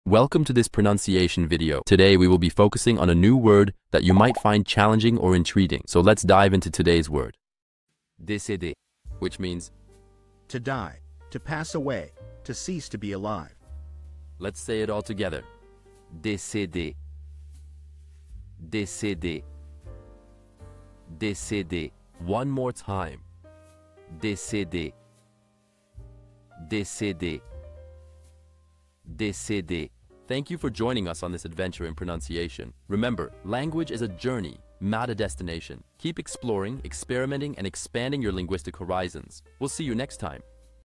How to pronounce Décéder
Master the Pronunciation of 'Décéder Which means die' - which means : To die;to pass away;to cease to be alive. 📝 with @BaguettePronunciation 🎓🗣️ - [Your Guide to French]

Learn to pronounce 'Décéder' perfectly in French with our easy and engaging tutorial by @BaguettePronunciation! 📖📽️ In this comprehensive video, we provide you with native speaker examples 🗣️👂 and phonetic breakdowns 🎵 to ensure you get the pronunciation just right.

How to say Die in French